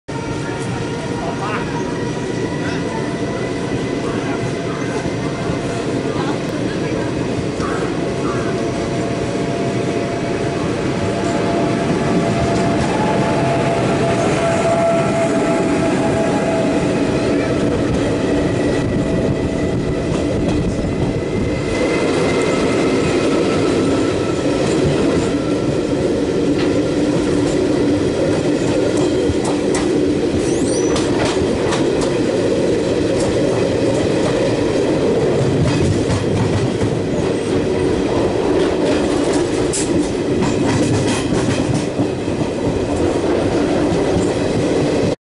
Indore Jodhpur Ranthambore Depart Bhawani Mandi, Overtake Tiger WAG7 Freight
WAP4 12465 Indore Jodhpur Ranthambore depart Bhawani Mandi, Overtake WAG7 BCNA Freight.

WAP4 Vadodara BRC 22705. Tiger face WAG7 Jhansi JHS 24520.

Bhawani Mandi, 101 km from Kota is in Jhalawar district of Rajasthan. It was established by Bhawani Singh, ruler of Jhalawar state in 1911 when the railway passed through this state. Platform here is divided between two states, Northern part is in Mandsaur district of Madhya Pradesh and the Southern part in Jhalawar district of Rajasthan. There have been many incidents where criminals took advantage of its location.

Bhawani Mandi is a famous mandi (market) for agriculture. Vegetables are specially cheap here. This place is also second, in orange production after Nagpur.

Ranthambore connects Indore and Jodhpur, covers 911 km. Belongs to North Western Railway.

Route / Junctions - Raikabag Palace, Pipar Road, Merta Road, Degana, Makrana, Phulera, Jaipur, Sawai Madhopur, Kota, Ramganj Mandi, Nagda, Ujjain, Dewas, Laxmibai Nagar.